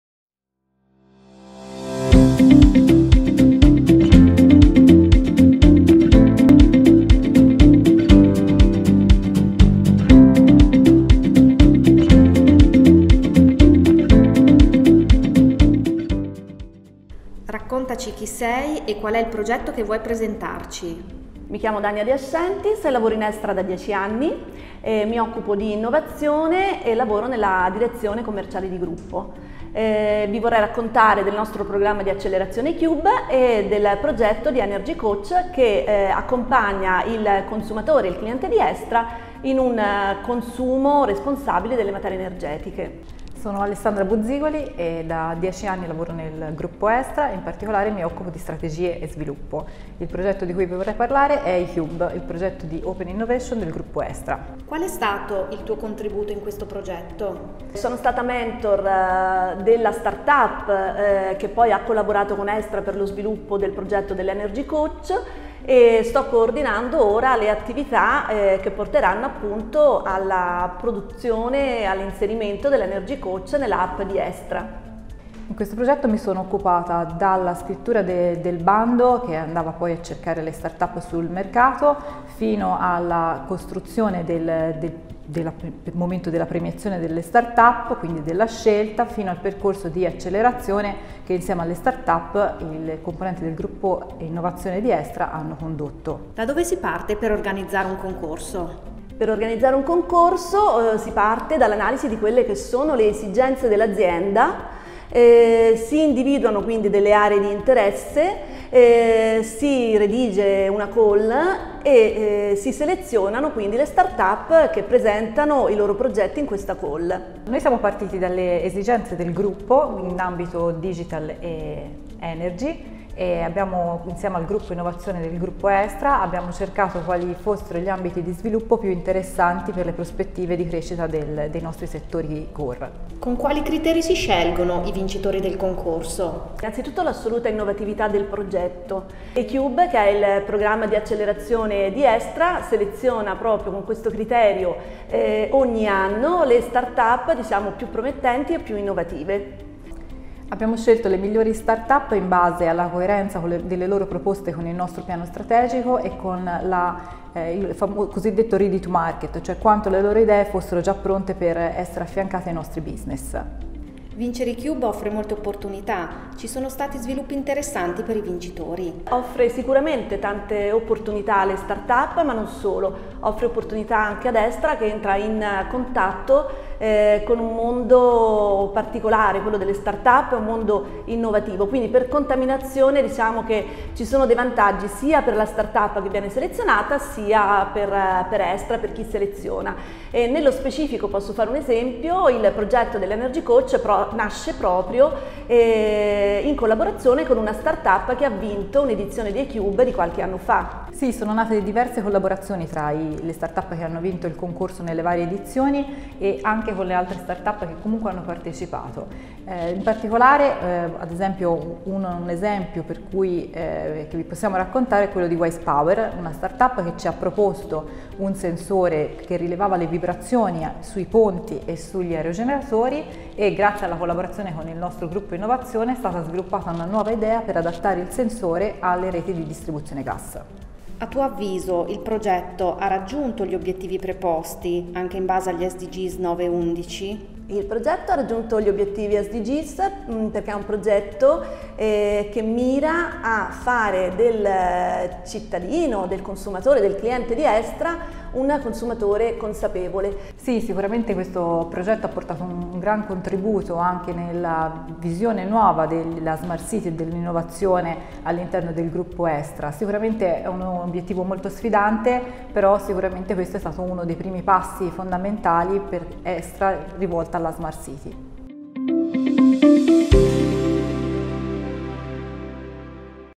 Voci di Estra - E-qube Startup&idea Challenge
Alessandra Buzzigoli e Dania De Ascentiis sono le protagoniste di questo primo appuntamento con Voci di Estra. Insieme ci presentano il progetto “E-qube Startup&idea Challenge”, la call per startup promossa da Estra, in collaborazione con l’acceleratore di startup Nana Bianca e StartupItalia.